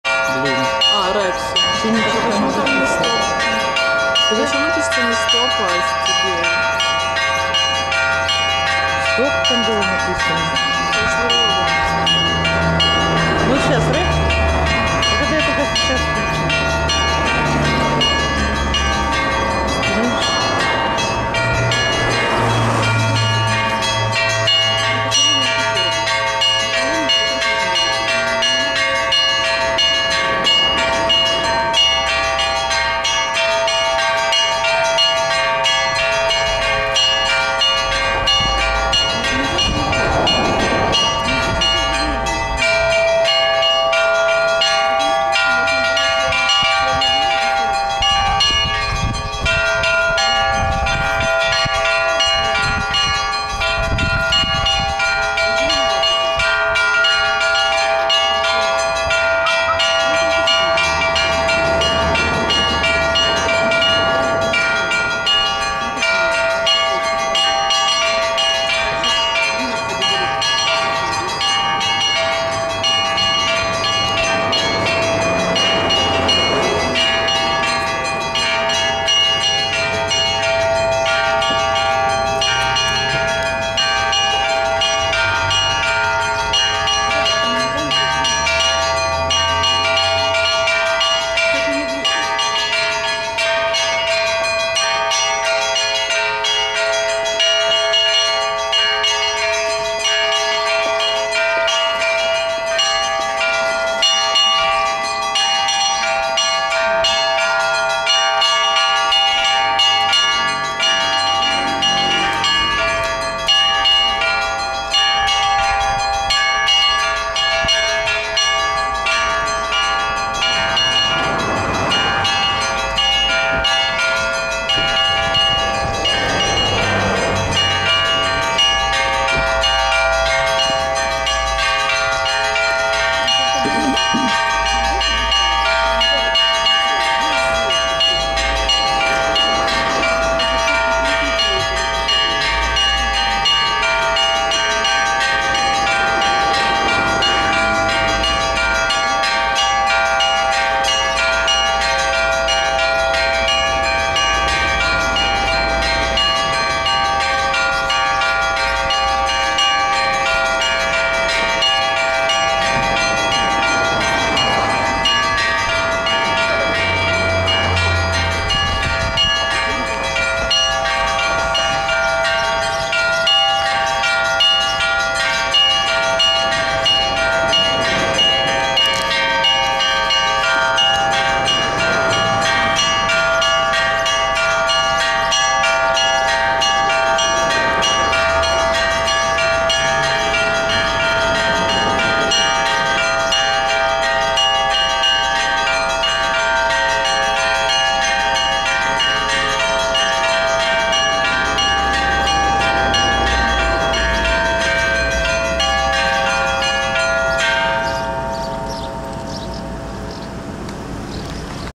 Колокольный звон в женском монастыре
Свято-Борисо-Глебский женский монастырь в Харьковской области: село Водяное Змиевского района. St. Boris and Gleb convent in the Kharkiv region: the village Vodyanoe Zmiev District.